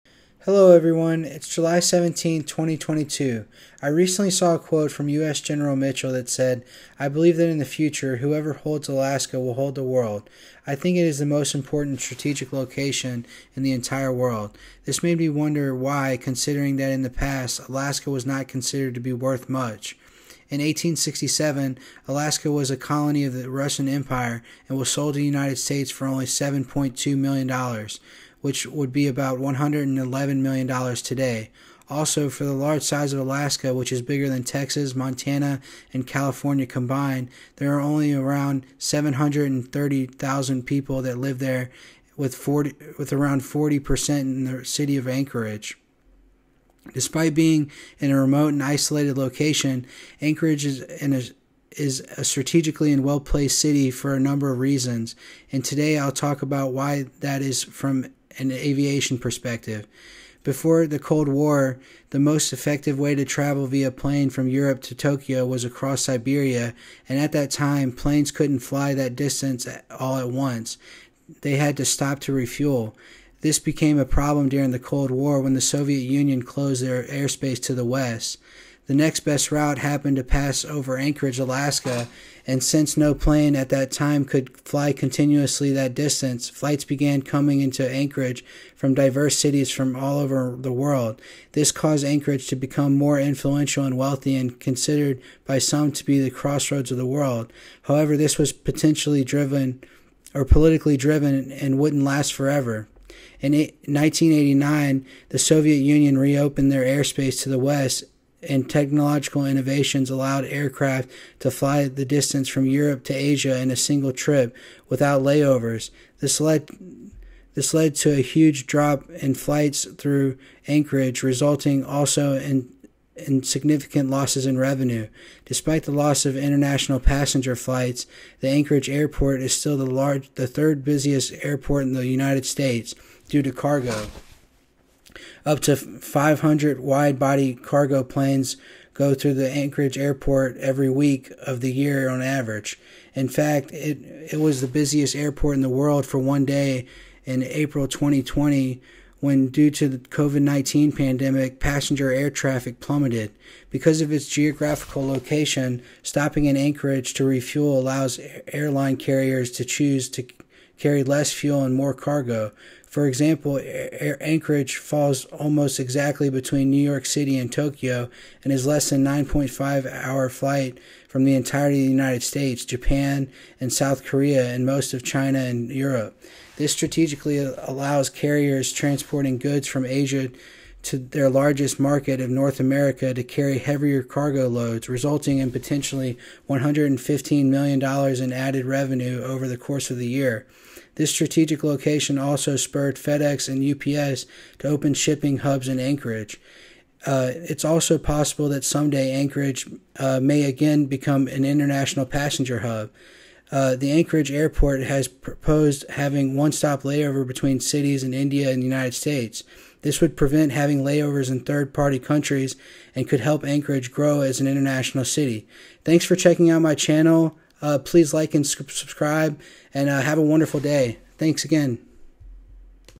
Why Alaska's Anchorage Airport is One of the Busiest
Despite being in a remote and isolated location, Anchorage is a strategically well-placed city for a number of reasons, and in this video, I’ll talk about why that is from an aviation perspective.

Anchorage became a stopover location for international flights to refuel during the Cold War when the Soviet Union closed their airspace to the West, which caused the city to become more influential and wealthy.

Today, even with few international passenger flights, Anchorage’s airport is still the third busiest airport in the United States due to cargo flights. Because of its geographic location, stopping in Anchorage to refuel allows airline carriers to choose to carry less fuel and more cargo.

It’s also possible that someday, Anchorage may again become an international passenger hub between countries like the United States and India, helping Anchorage grow into a global city.

Thanks for checking out my channel. Have a great day!